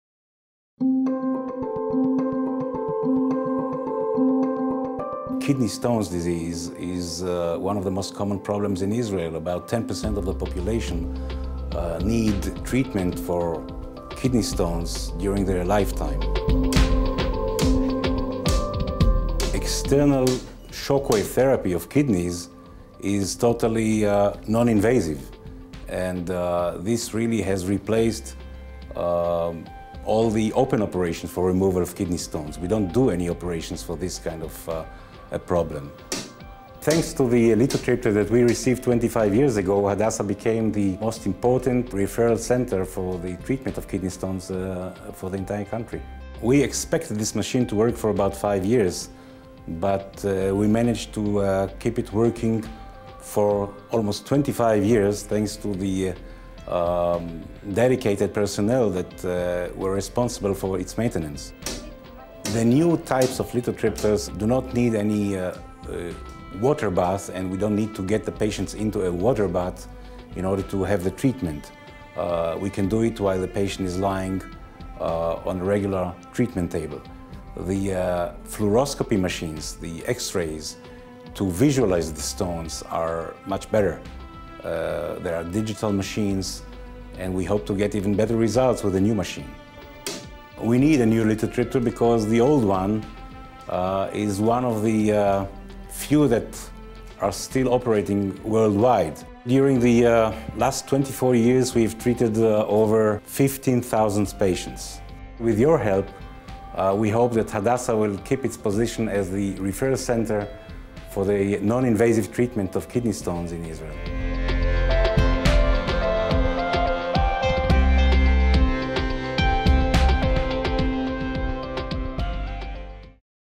Hadassah's Lithotripsy Unit -- Kidney Stones
Help keep Hadassah current in kidney treatment with the latest noninvasive equipment to eradicate kidney stones.